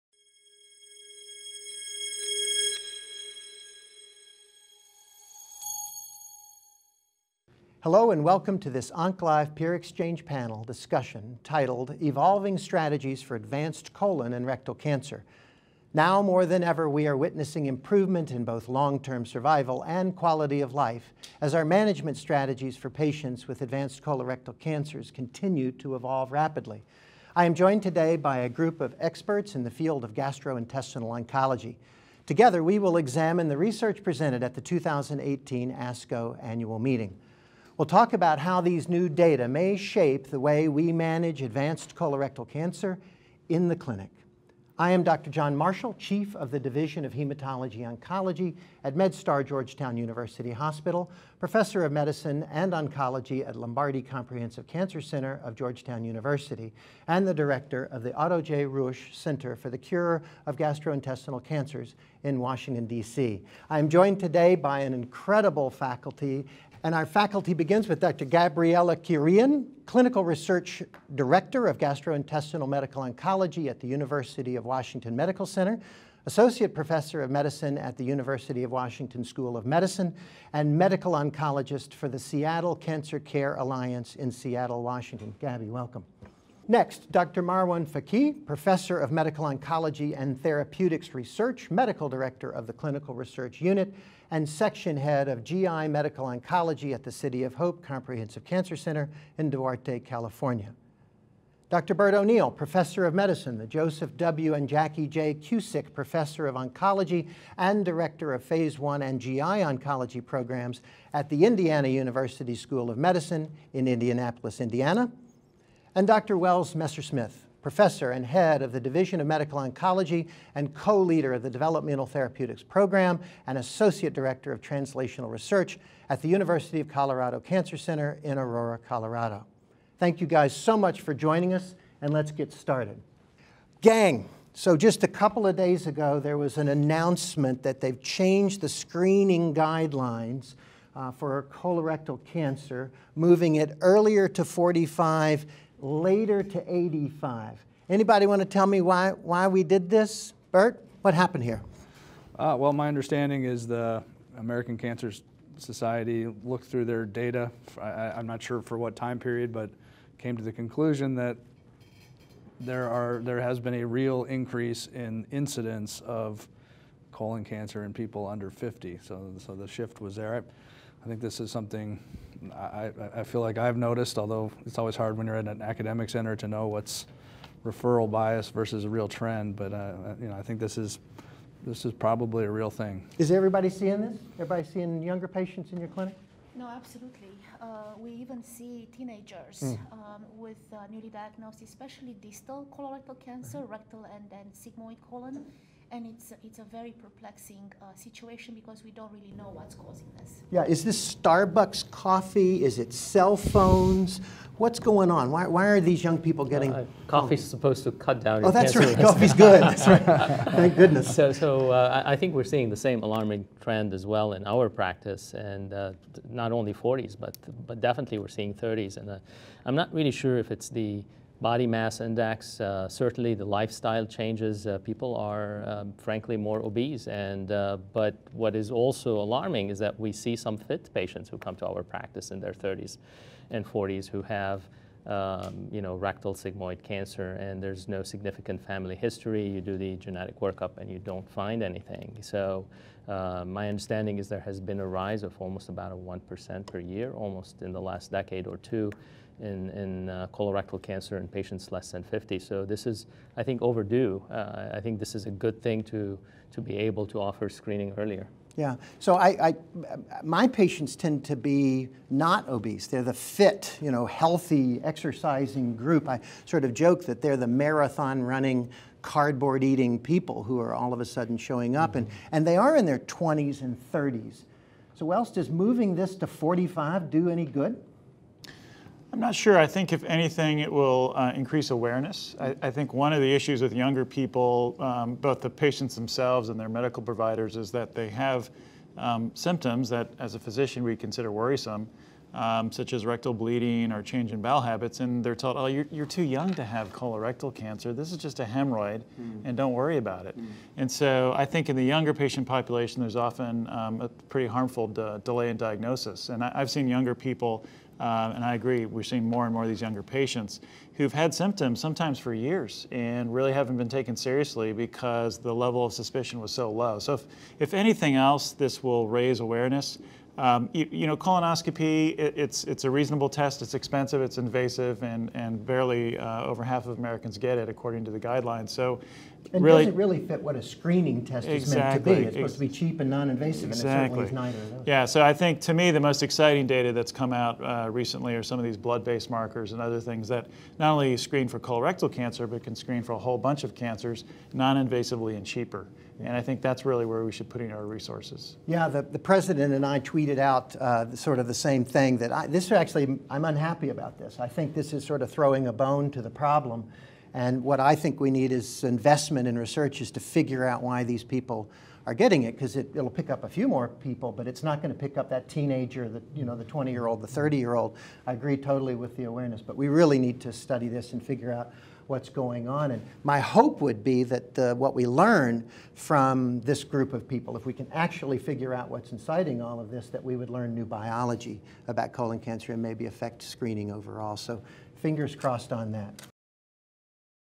Updates in Screening Guidelines for CRC
John L. Marshall, MD; Gabriela Chiorean, MD; Marwan Fakih, MD; Bert H. O’Neil, MD; and Wells A. Messersmith, MD, FACP, discuss the changes in screening guidelines for colorectal cancer, based on the increasing trend of incidence in adults under the age of 50.